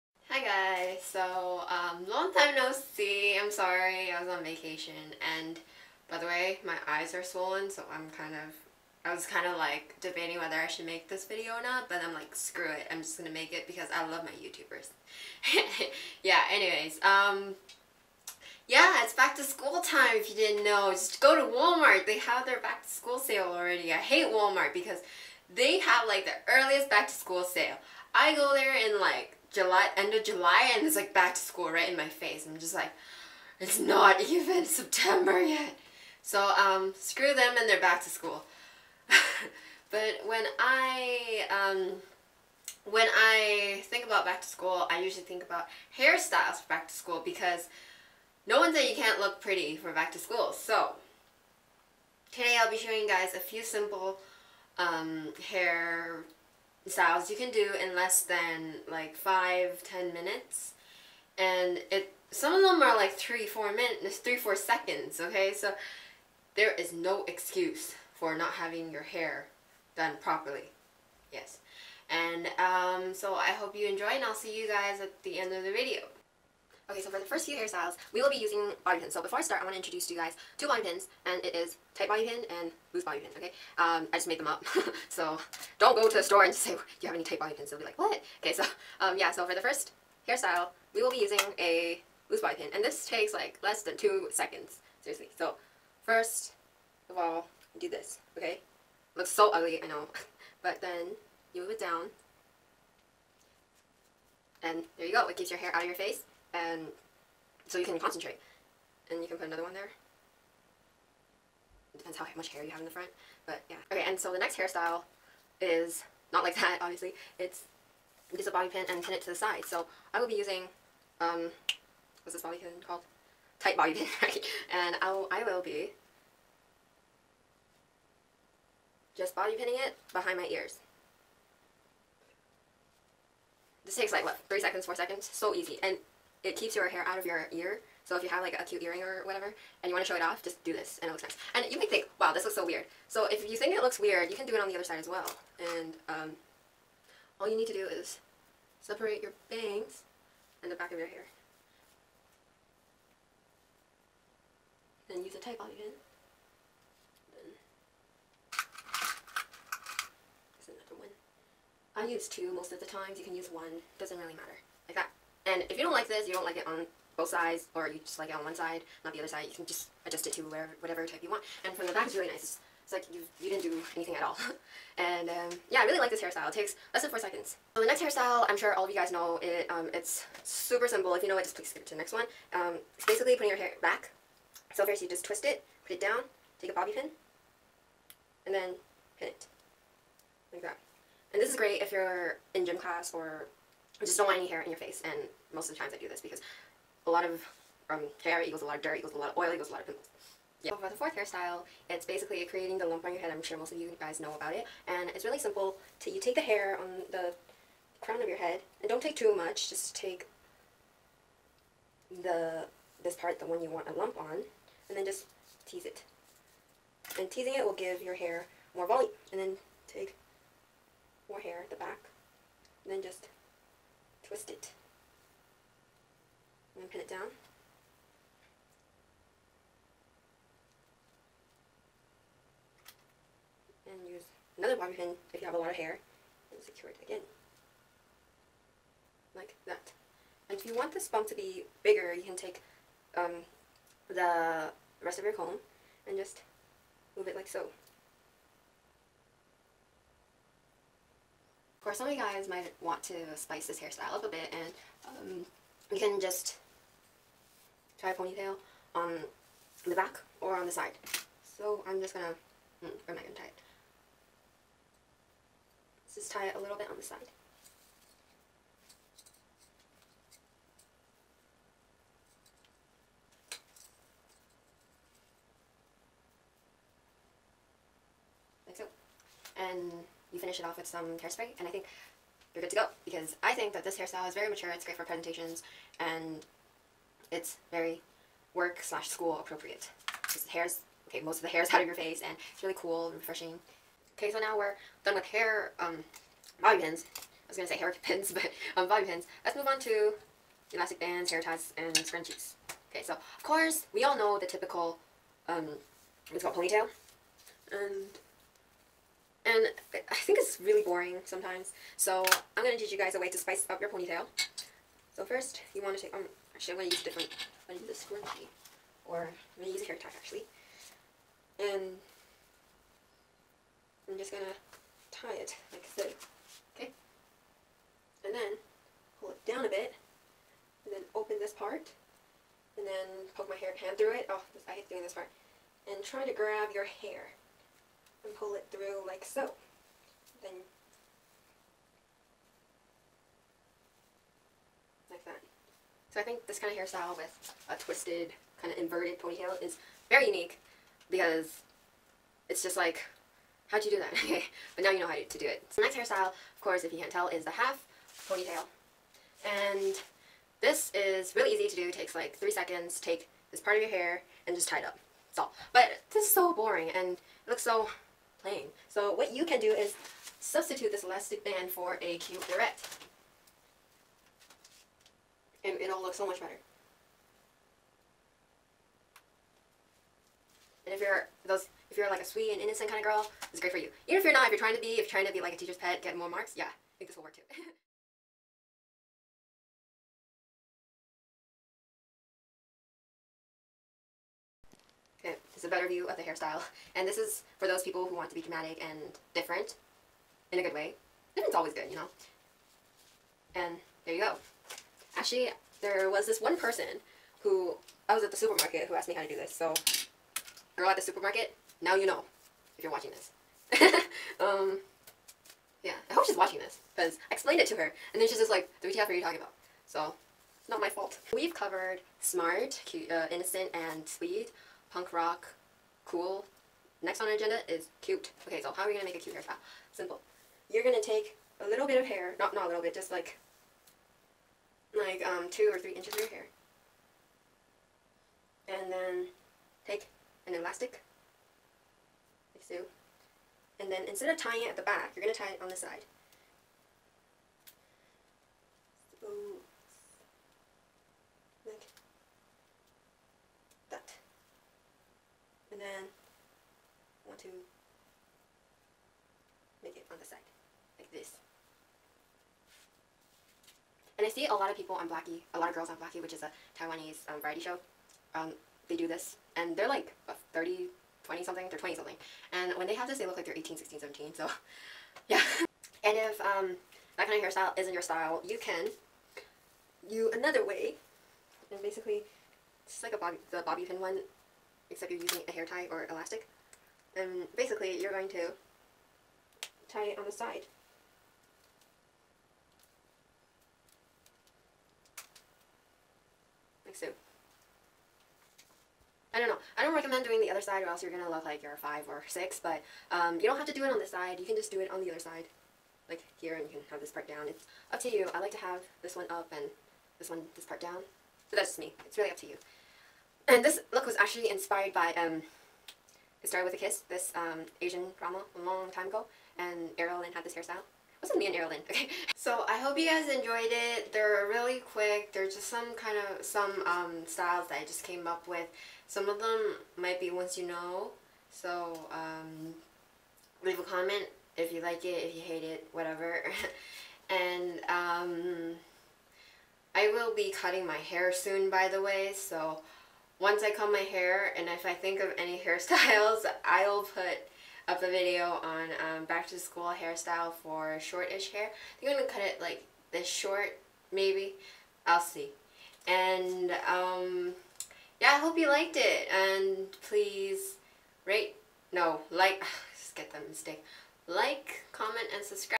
10 Back to school Hairstyles in less than 5 minutes
HI FRIENDS! so here are some back to school hair styles i just thought of, most of them im sure you guys know about already but if you do please skip to the next one. (: HOPE YOU GUYS LIKE IT!

What you need:
-bobby pins
-hair ties
-comb
-clips